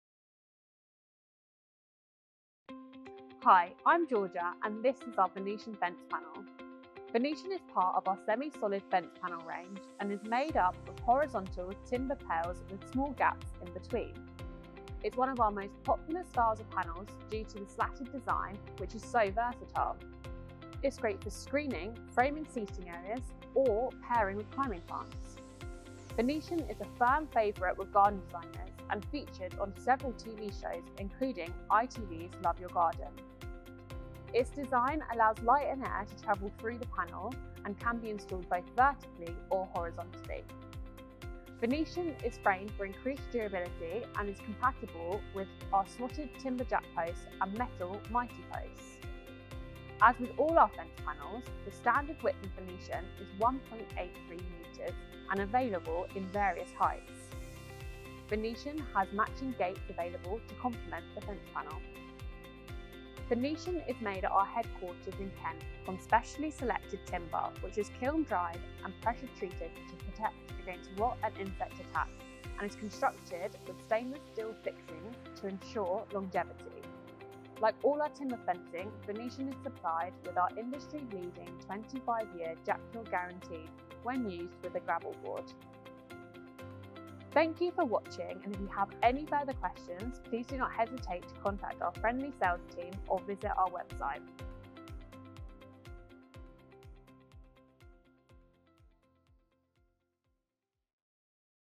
Venetian Fence Panel Review | Jacksons Fencing
Discover the elegance and durability of our semi-solid Venetian Fence Panels, perfect for adding a touch of contemporary style to your garden. Crafted from premium timber and backed by a 25-year guarantee, these high-quality semi-solid panels are ideal for creating a beautiful outdoor space.

🔹 Premium Quality Timber: Our panels are manufactured using the finest materials, ensuring they stand the test of time.
🔹 25-Year Guarantee: Enjoy peace of mind with our extensive Jakcure guarantee.
🔹 Stylish Design: The sleek, modern look of our Venetian Fence Panels enhances any garden setting.
🔹 Perfect for Screening: Provide segmentation and an attractive backdrop for your garden.

Upgrade your outdoor space today! Watch the video to see how easy it is to install and the stunning transformation our Venetian Fence Panels can bring to your garden.